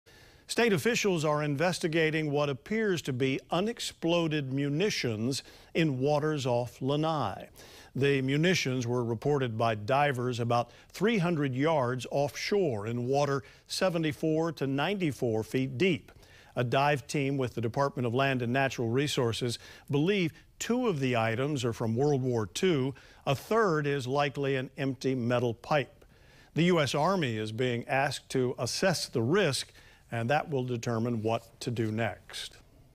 DLNR investigates unexploded ordnances off Lanai
The Department of Land and Natural Resources (DLNR) sent aquatic biologists from the Division of Aquatic Resources (DAR) to conduct rapid dive assessments of three suspected unexploded ordnances (UXO) off Lanai's south shore.